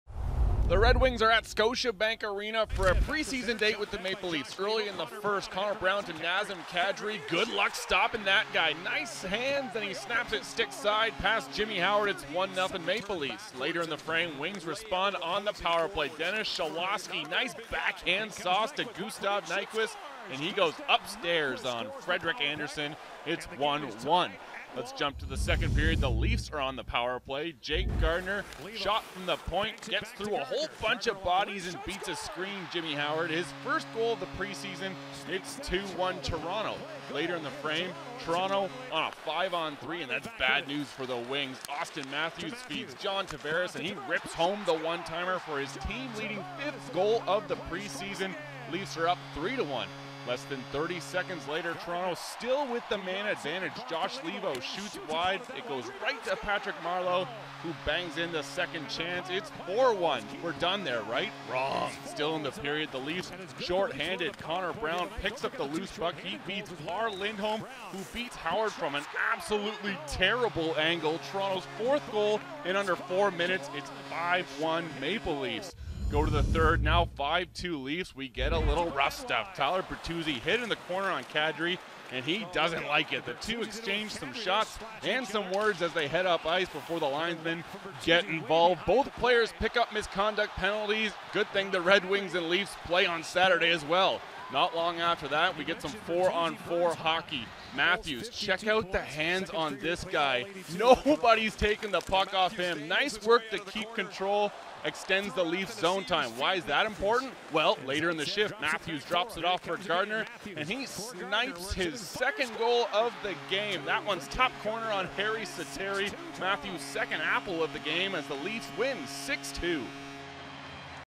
NHL Pre-season Highlights | Red Wings vs. Maple Leafs - Sept. 28, 2018
Jake Gardiner scored a pair of goals to help the Toronto Maple Leafs defeat the Detroit Red Wings 6-2 in the pre-season.